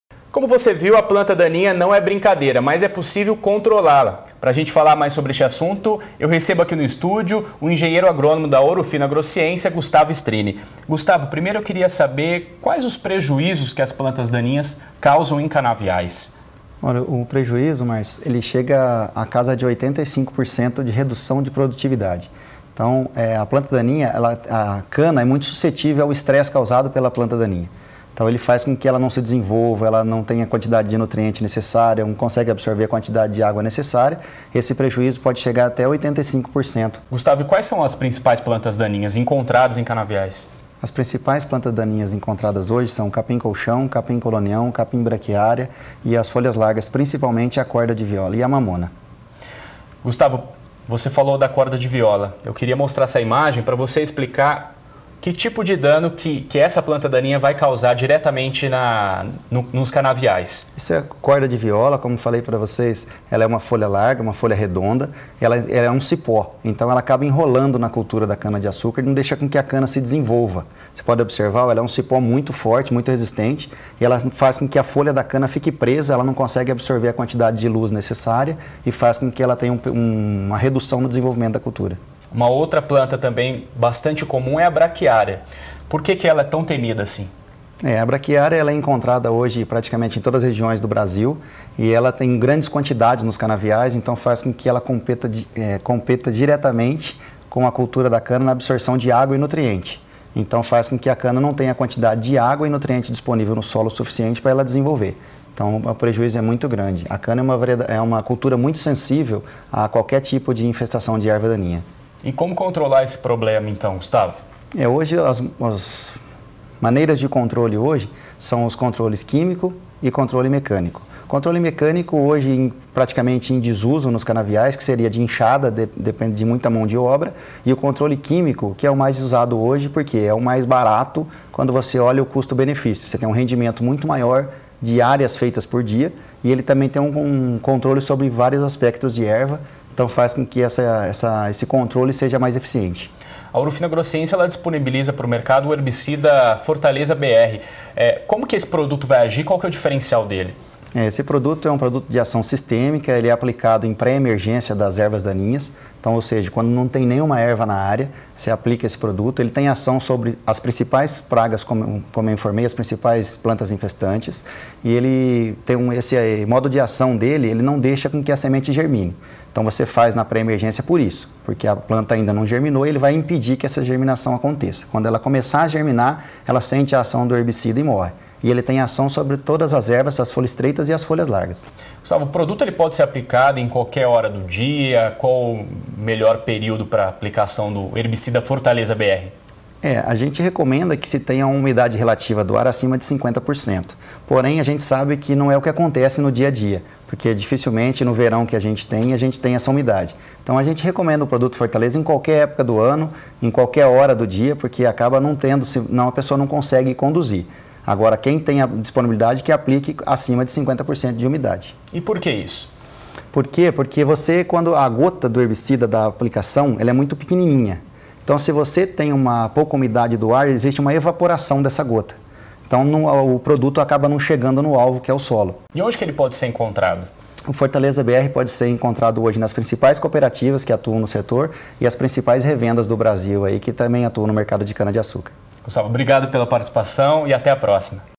06/10/2014 - Fortaleza BR contra as plantas daninhas - Ourofino em Campo
O engenheiro agrônomo da Ourofino Agrociência, Gustavo Strini, fala sobre o uso do herbicida Fortaleza BR para o controle das plantas daninhas na cana-de-açúcar.